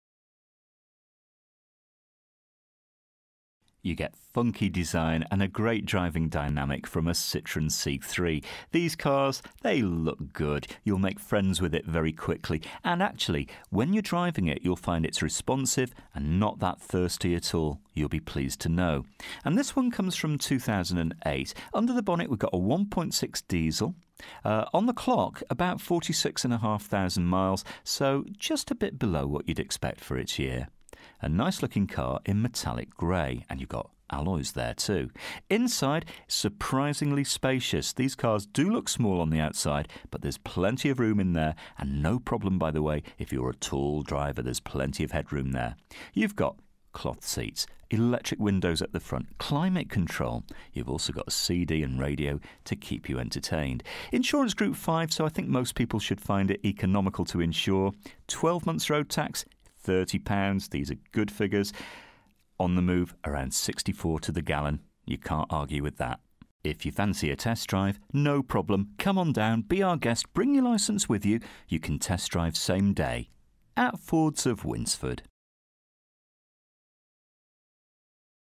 USED CITROEN C3 DIESEL HATCHBACK (2008) 1.6 HDI 16V EXCLUSIVE 5DR - PK08BHL
CITROEN C3 DIESEL HATCHBACK (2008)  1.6 HDI 16V EXCLUSIVE 5DR

DETAILS:
First registered: 14/03/2008
Current mileage: 46,540
Registration: PK08BHL
Colour: GREY

FEATURES:
front electric windows, remote central locking, cloth interior, all round airbags, climate control, eps, abs, alloy wheels, metallic paint, cd, parcel shelf present, hand book present